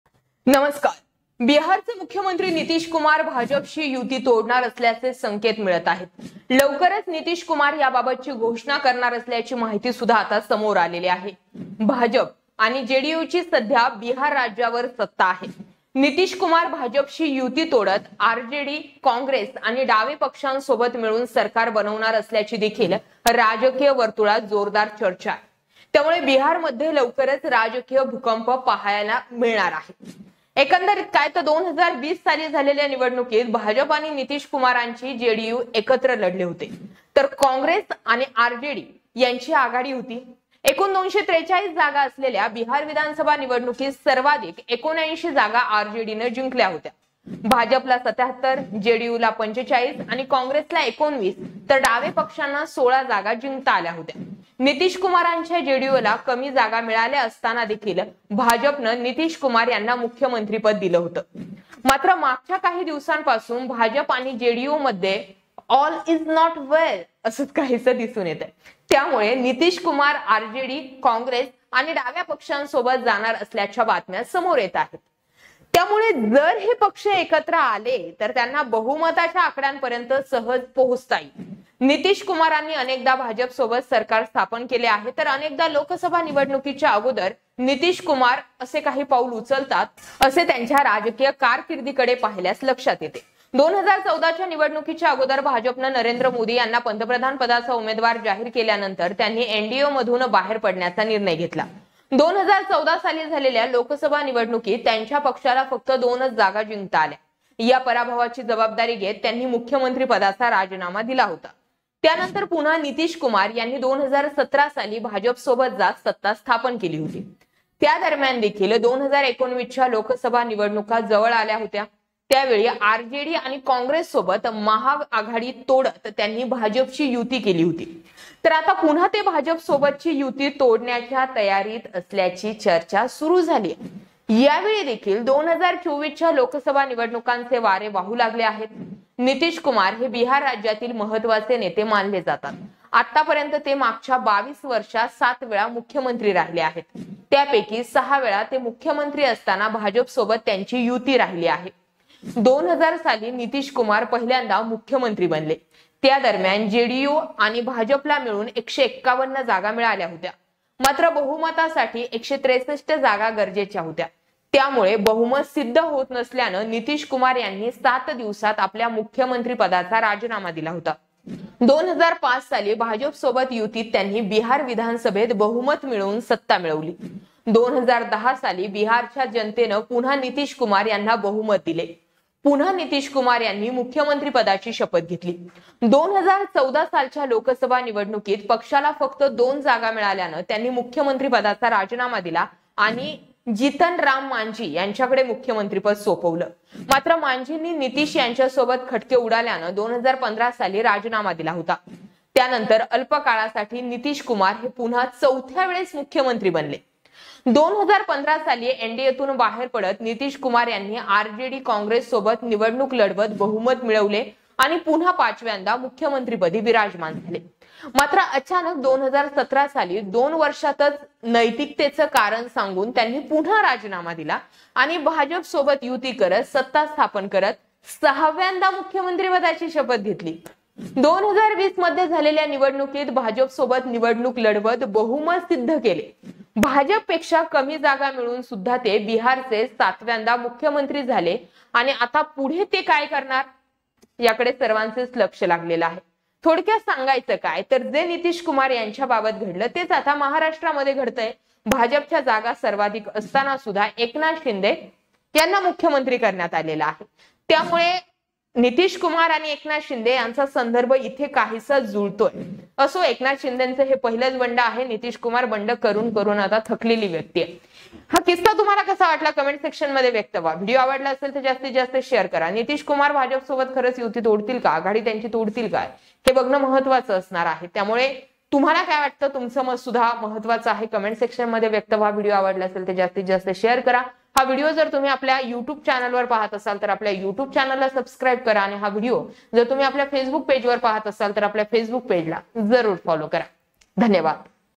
नितीश कुमारांच्या राजकारणाचा असा आहे पॅटर्न ! Nitish Kumar And Tejashwi Yadav Eknath Shinde Shivsena
There are indications that Bihar Chief Minister Nitish Kumar will break the alliance with BJP. Information has come to light that Nitish Kumar will soon make an announcement about this. BJP and JDU are currently in power in the state of Bihar. There is also a strong discussion in political circles that Nitish Kumar will break the alliance with BJP and form the government with RJD, Congress and Left parties. So this is the talk that we will soon see a political earthquake in Bihar. BJP and Nitish Kumar's JDU had fought together in the elections held in 2020. Congress and RJD were leading. RJD won maximum 79 seats in the Bihar assembly elections out of total 243 seats. BJP could win 77 seats, JDU 45, Congress 19 and Left parties 16. Even when Nitish Kumar's JDU won fewer seats, the BJP gave Nitish Kumar the chief ministership. However, from the last few days, it is being seen that all is not well between BJP and JDU. So there are reports that Nitish Kumar will go with RJD, Congress and Left parties. So if these parties come together, they can easily reach the majority. Nitish Kumar has often formed the government with the BJP. If we look at his political career, Nitish Kumar often takes such a step before the Lok Sabha elections. Ahead of the 2014 elections, Narendra Modi had decided to quit the NDA after the BJP announced it as its prime ministerial candidate. In the 2014 Lok Sabha elections, his party could win only two seats. Taking responsibility for this defeat, he resigned from the post of Chief Minister. After that again, Nitish Kumar formed the caste government with BJP in 2017. Meanwhile, the 2019 Lok Sabha elections were approaching. At that time, breaking the grand alliance with RJD and Congress, he formed an alliance with BJP. Now again there is talk that he is preparing to break the alliance with BJP. This time also the winds of 2024 Lok Sabha elections have started blowing. Nitish Kumar is considered to be an important leader in the state of Bihar. So far he has been Chief Minister 7 times in last 22 years. He has been in alliance with the BJP for 6 times when he was the Chief Minister. In 2000, Nitish Kumar became the Chief Minister for the first time. Meanwhile, JDU and BAPJ together got 151 seats. However, 163 seats were required for majority. Nitish Kumar resigned from his post as Chief Minister in 7 days as the majority was not proven. In 2010, the people of Bihar again gave majority to Nitish Kumar. Again Nitish Kumar took oath as Chief Minister. In the 2014 Lok Sabha elections, the party got only two seats, so he resigned from the post of Chief Minister and handed over the post of Chief Minister to Jitan Ram Manjhi. However, Manjhi resigned in the year 2015 after falling out with Nitish. After that Nitish Kumar again became the Chief Minister for the fourth time for a short time.
In 2015, Nitish Kumar came out of the NDA, contested the elections with RJD, Congress and won the majority and became the Chief Minister again for the fifth time. But suddenly in 2017, within two years, he resigned again citing moral reasons. And formed an alliance with the BJP and took oath as Chief Minister for the sixth time. In the elections held in 2020, he contested the election with the BJP and proved his majority. He became the Chief Minister of Bihar for the seventh time despite winning less seats than the BJP. Now everyone's attention is on what's next.

MarathiMandali uddhav thackeray mahavikas aghadi maha vikas aghadi bihar poltics bihar politics nitish kumar